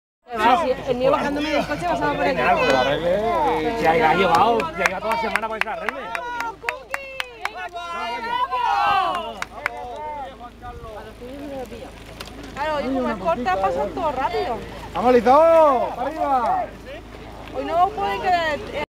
BTT LETUR 2012.SEGUNDOS KILÓMETROS I.PRODEPOR
SEGUNDO PASO EN LA PRUEBA DE BTT DISPUTADA EN LETUR EL 27/05/2012 DENTRO DEL VI CIRCUITO DE BTT ORGANIZADO POR LA DIPUTACIÓN PROVINCIAL DE ALBACETE.